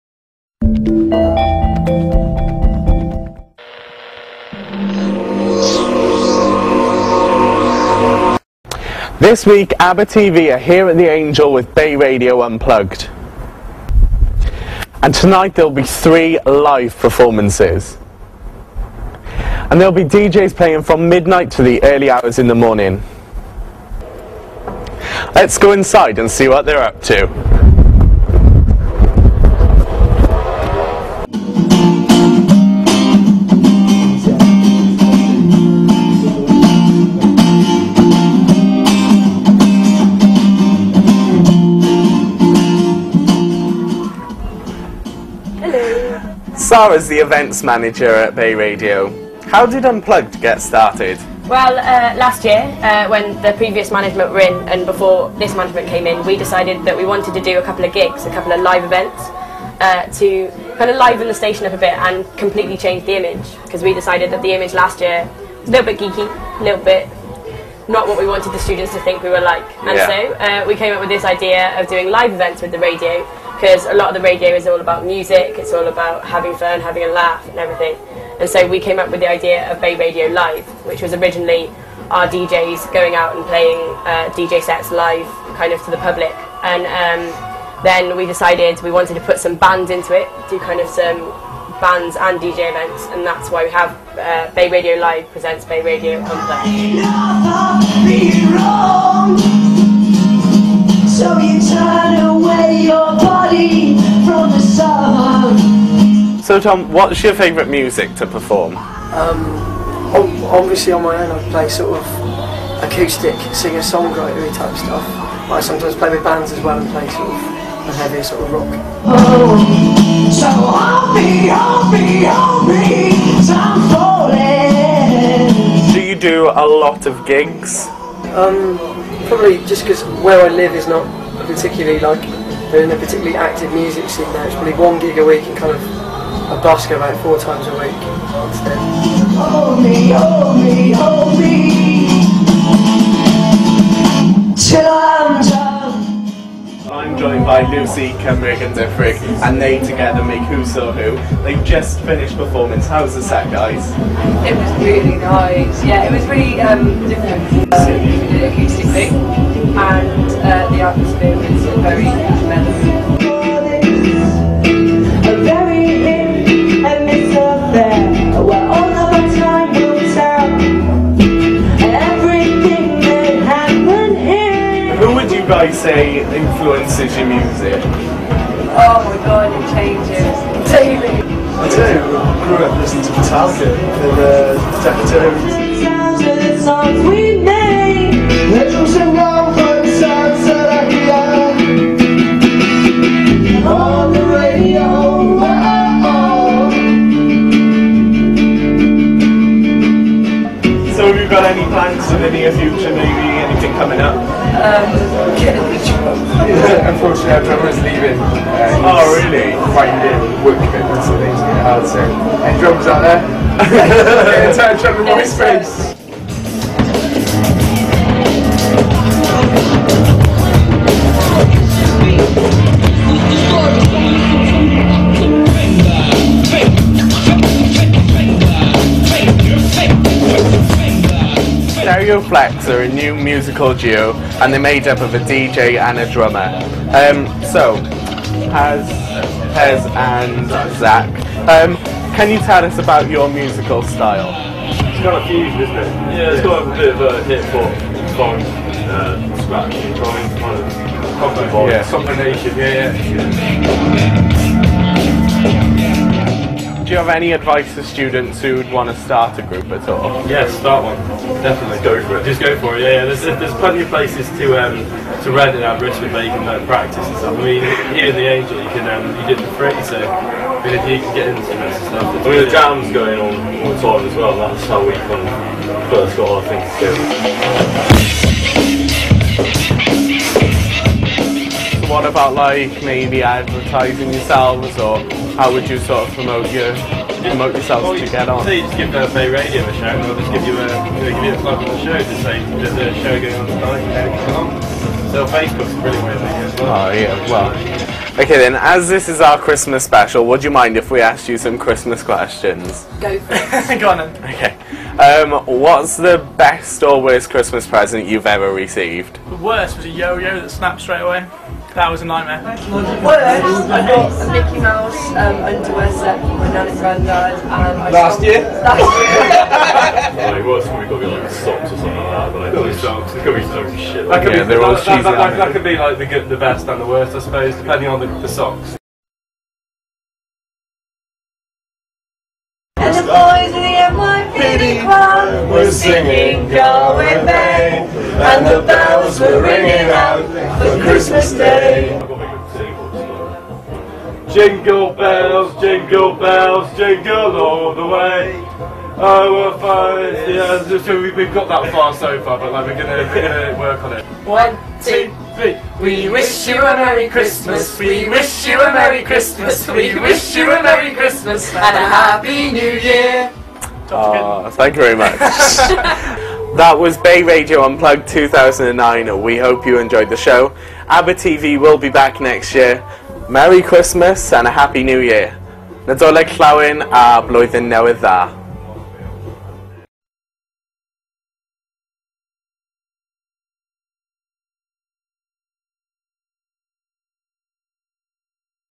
Aber TV presents Bay Radio Unplugged
Aber TV attended BayRadio Unplugged at The Angel in Aberystwyth, featuring Thom Morecroft, Who Saw Who and Stereoflex
(2009)